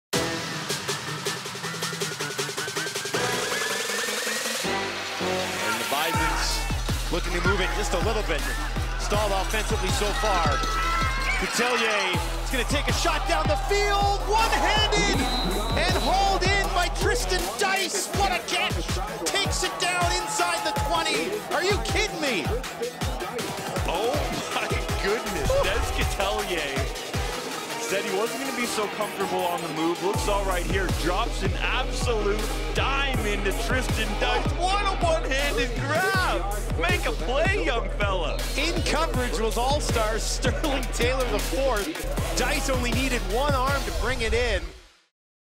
Trysten Dyce Ridiculous One-Hander vs CGY | Pizza Pizza Play of the Week (Oct 29-Nov 4)
Dyce pulls down a crazy one-handed, sideline catch in the Bisons Hard Cup semifinal against the Calgary Dinos.